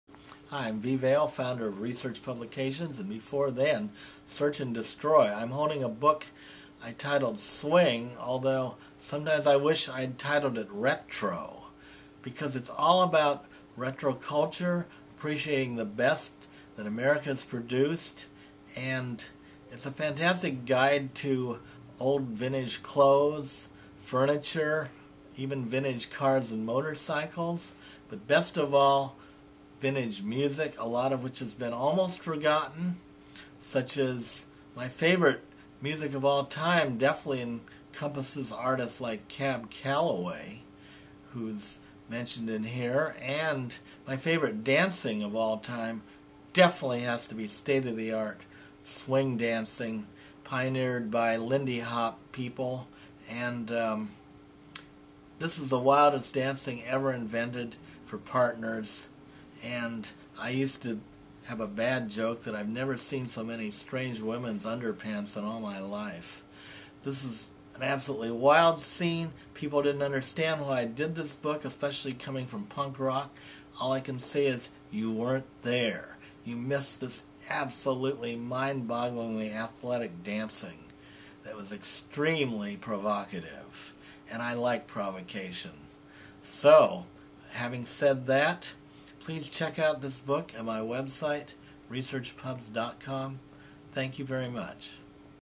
V. Vale on "Swing: The New Retro Renaissance" book
V. Vale of RE/Search Publications, on "Swing: The New Retro Renaissance" book. Dozens of interviews about swing dancing, clubs, vintage clothing and styling and much more!

Order directly from the publisher! Support independent publishing!